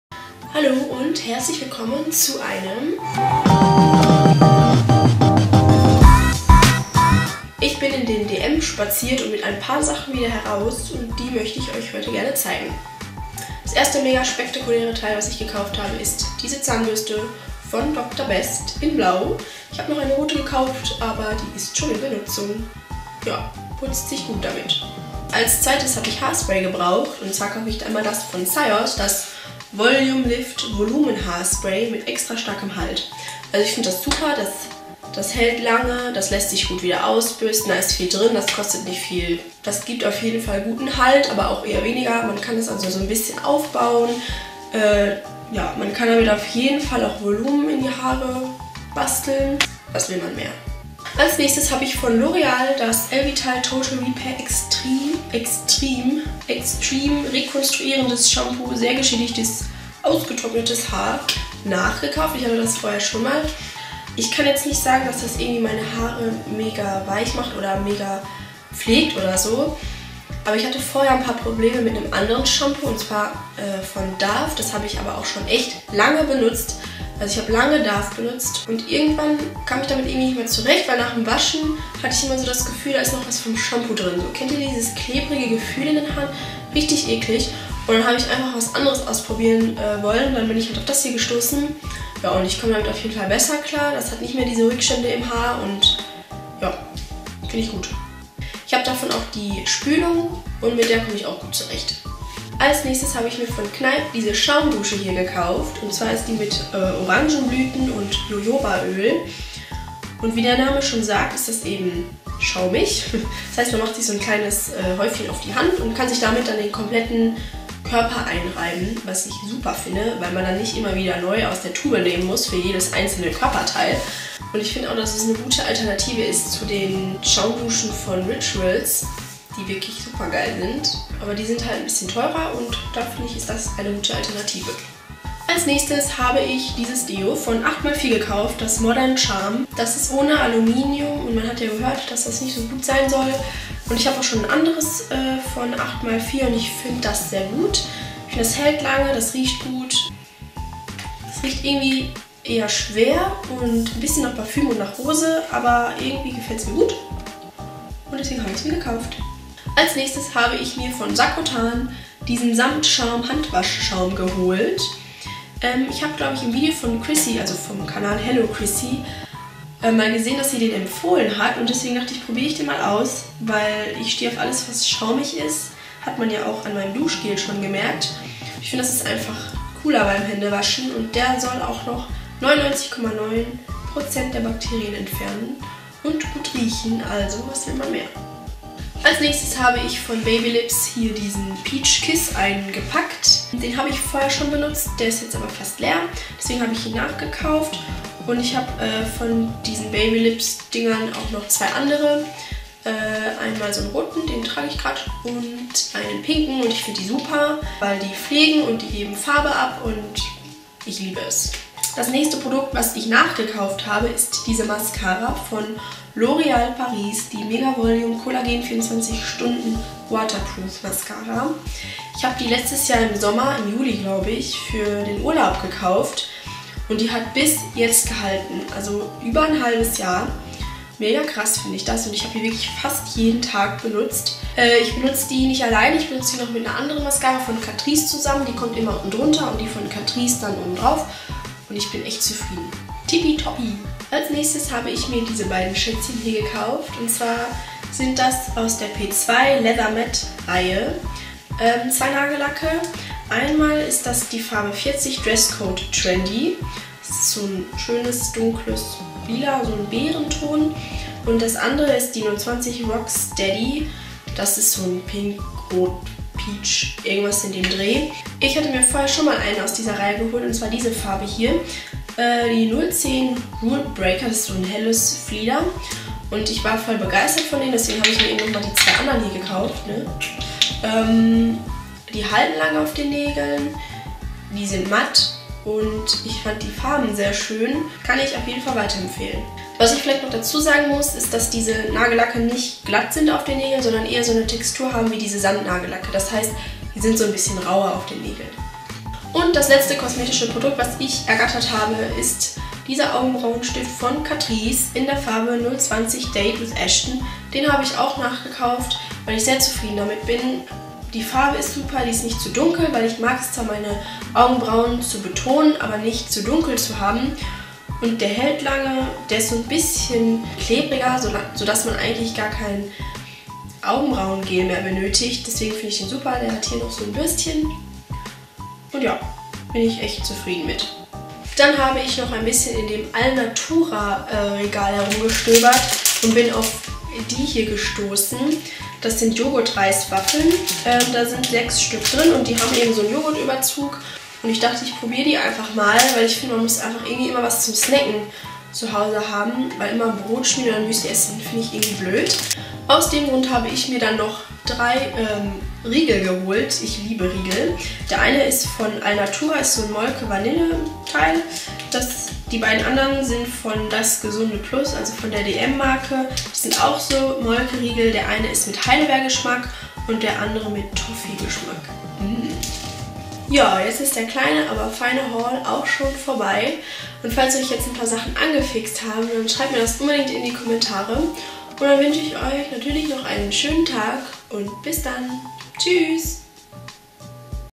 dm Haul | MrsHanala
Viel Spaß bei meinem dm Haul!
Leider lassen die Lichtverhältnisse (und der Nagellack auf meinen Nägeln) zu wünschen übrig, verzeiht mir das bitte ;)

Wie immer freue ich mich sehr über konstruktive Kritik :)